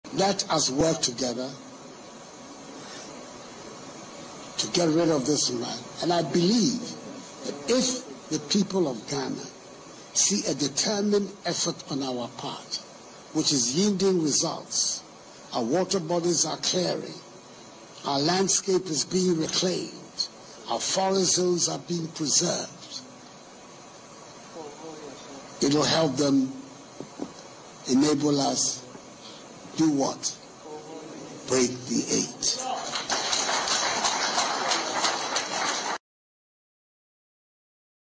Increased effort against galamsey will convince Ghanaians to help us 'break the eight' - Nana Addo
The President, Nana Akufo-Addo has told his Metropolitan, Municipal and District Chief Executives (MMDCEs) that a concerted effort to fight galamsey will convince Ghanaians to help the New Patriotic Party (NPP).